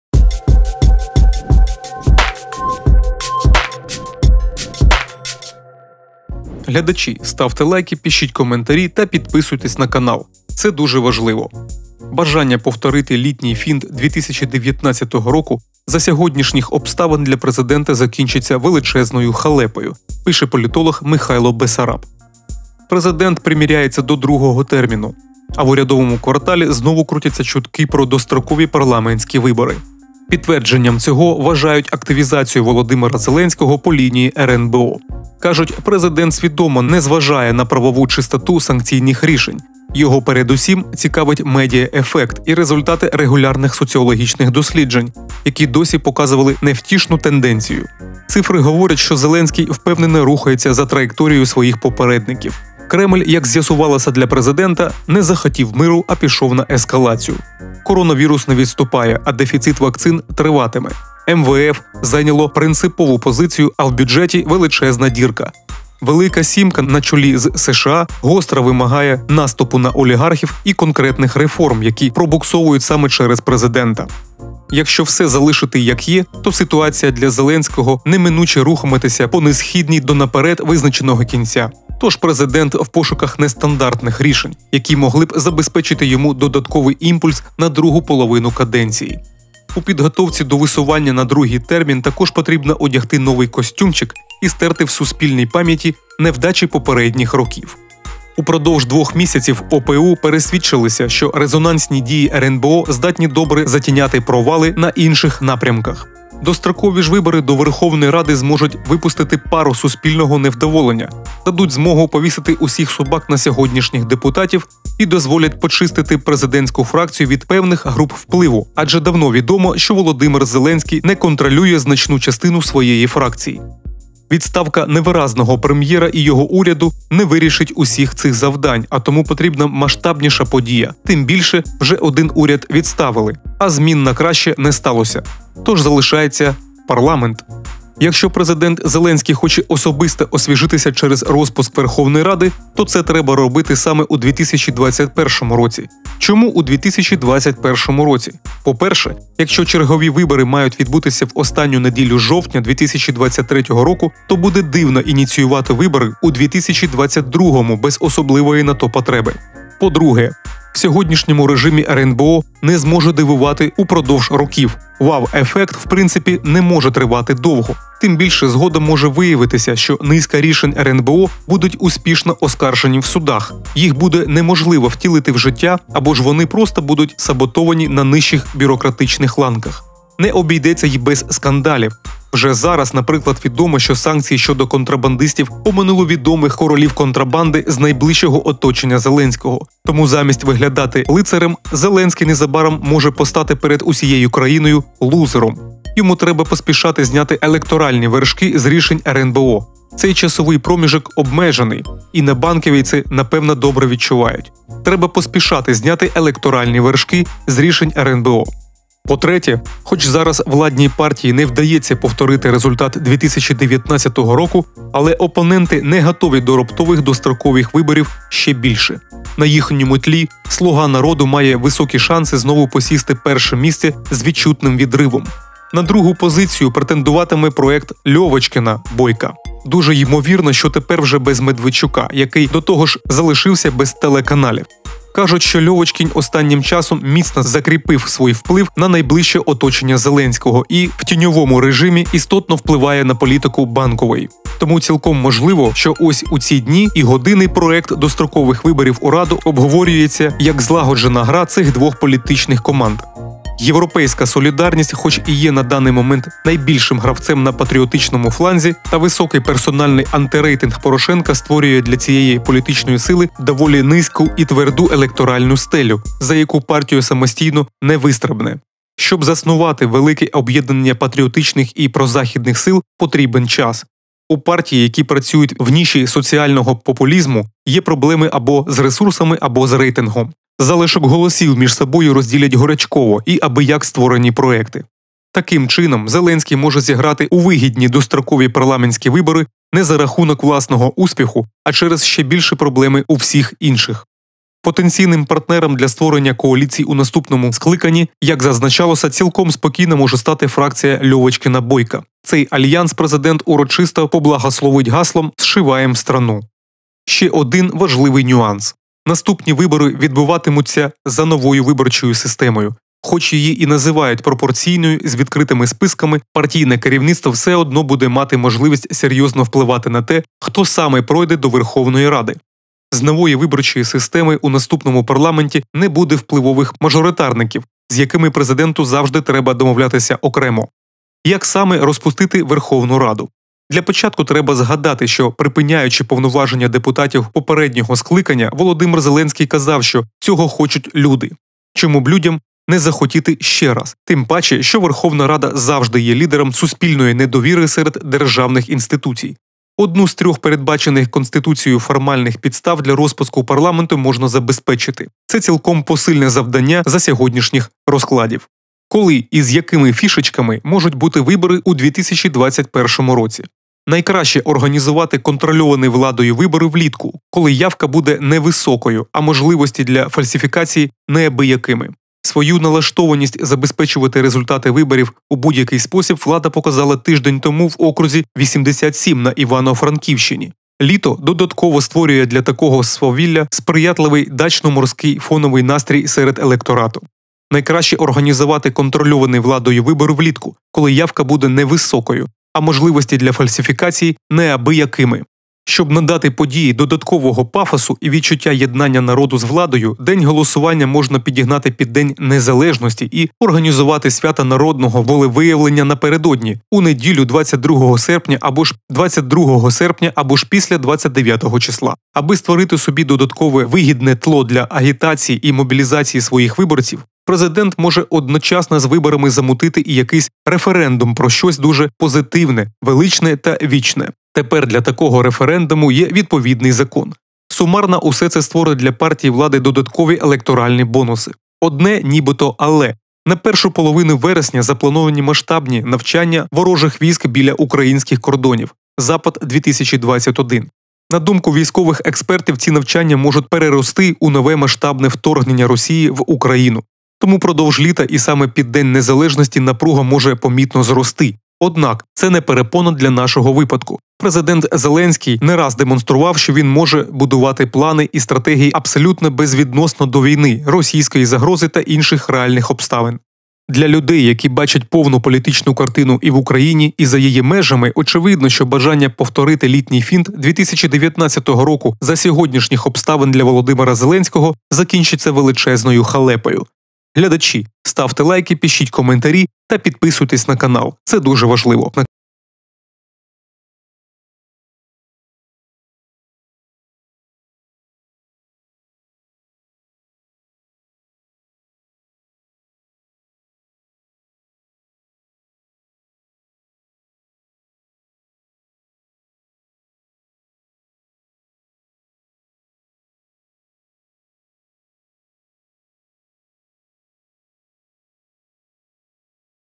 Халепа! У Зеленського цейтнот - Розпустити Раду. Наступні депутати - зброд. Забрати мандати
Бажання повторити літній фінт 2019 року за сьогоднішніх обставин для президента закінчиться величезною халепою, пише політолог Михайло Бесараб

Президент приміряється до другого терміну, а в урядовому кварталі знову крутяться чутки про дострокові парламентські вибори. Підтвердженням цьому вважають активізацію Володимира Зеленського по лінії РНБО. Кажуть, президент свідомо не зважає на правову чистоту санкційних рішень. Його передусім цікавить медіа-ефект і результати регулярних соціологічних досліджень, які досі показували невтішну тенденцію. Цифри говорять, що Зеленський впевнено рухається за траєкторією своїх попередників.